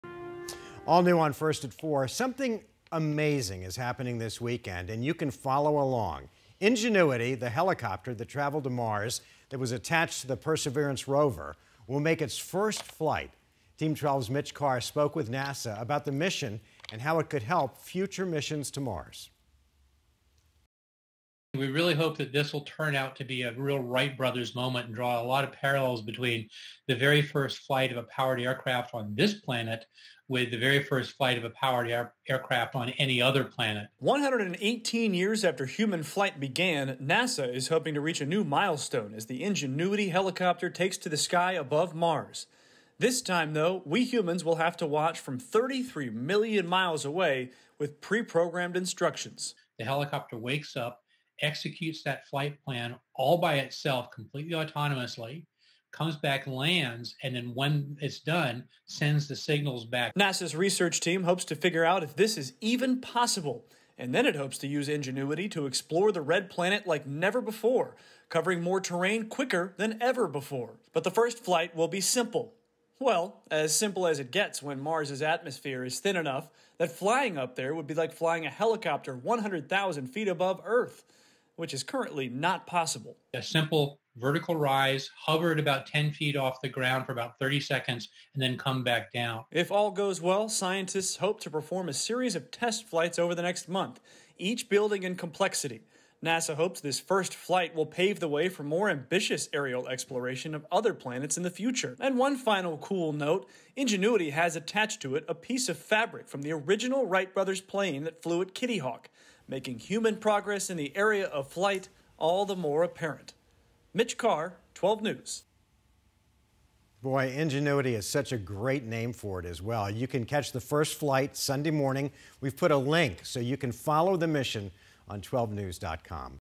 NASA’s Ingenuity helicopter scheduled to make first flight on Mars
Sunday marks a huge day in the history of space exploration and in the history of human flight, which began with the Wright Brothers in 1903.

NASA’s Ingenuity helicopter is scheduled to make its first flight on Mars, making it the first powered flight on another planet.

The helicopter traveled to Mars attached to the bottom of the Perseverance rover, which landed on the red planet in February.